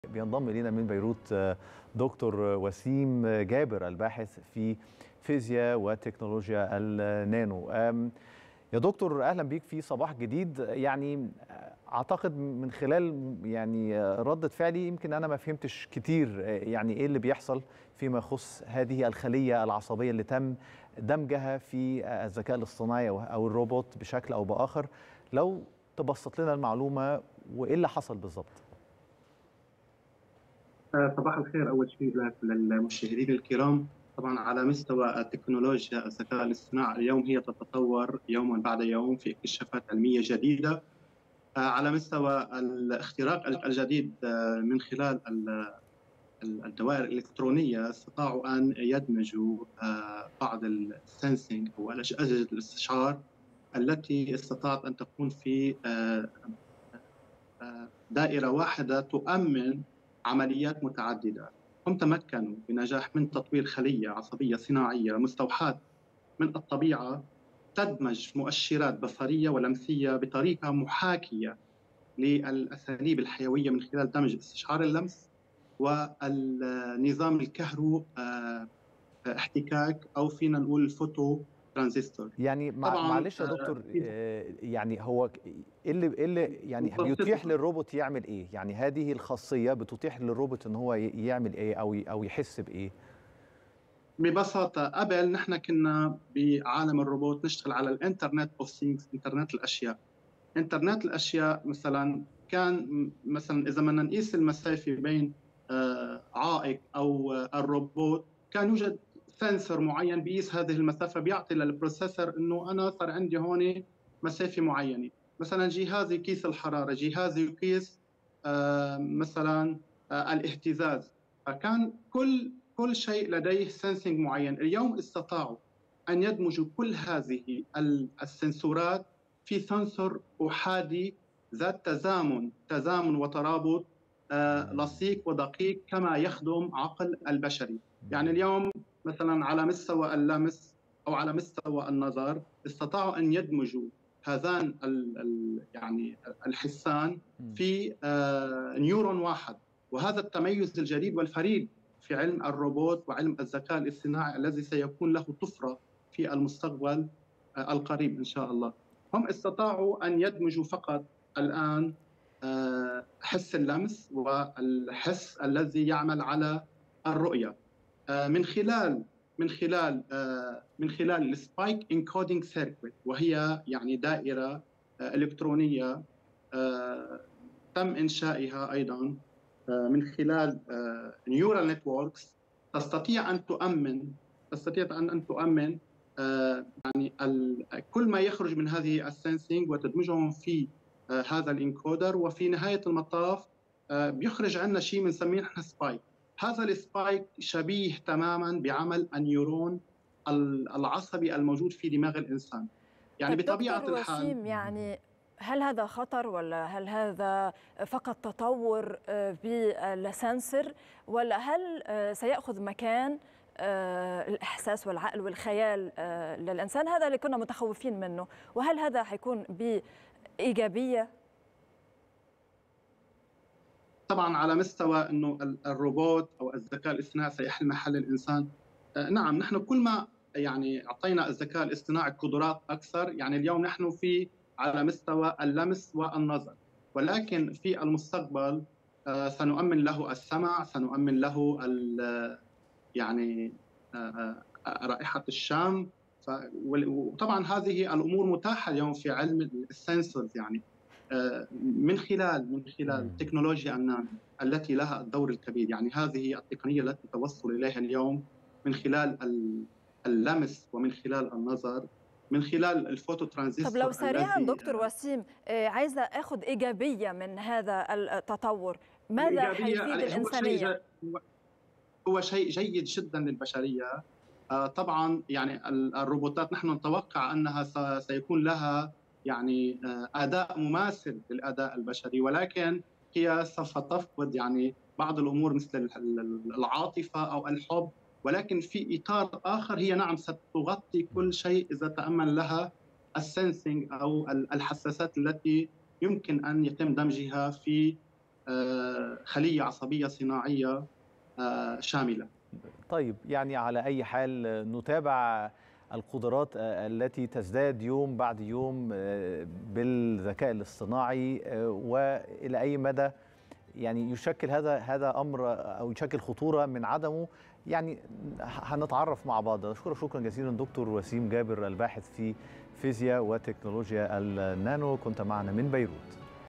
تطوير خلية عصبية اصطناعية لتعزيز قدرات الذكاء الاصطناعي.. التفاصيل الكاملة مع د. وسيم جابر
تطوير خلية عصبية اصطناعية لتعزيز قدرات الذكاء الاصطناعي.. التفاصيل الكاملة مع د. وسيم جابر الباحث في تكنولوجيا النانو

18/9/2023